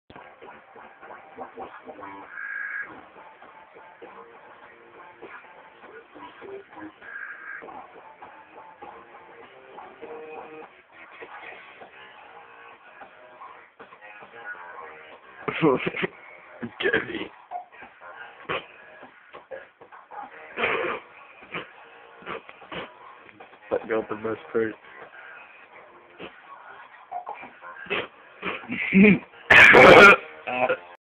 Buddy dancin hard while mixing in his basement
Hardcore dj to the max mixing in his basement doin some pretty funny moves. Not the best quality but you still get the point